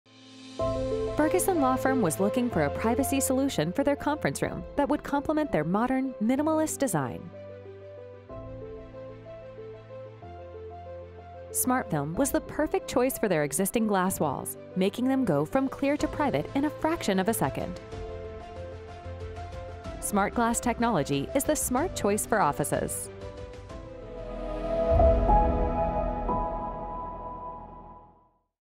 Smart Film In a Law Office
Watch smart film go from clear to frosted on demand in this conference room, providing instant privacy.

Smart glass technology was the perfect solution for Ferguson Law Firm, a company that was looking to create a modern, open connected office space while maintaining privacy during confidential meetings.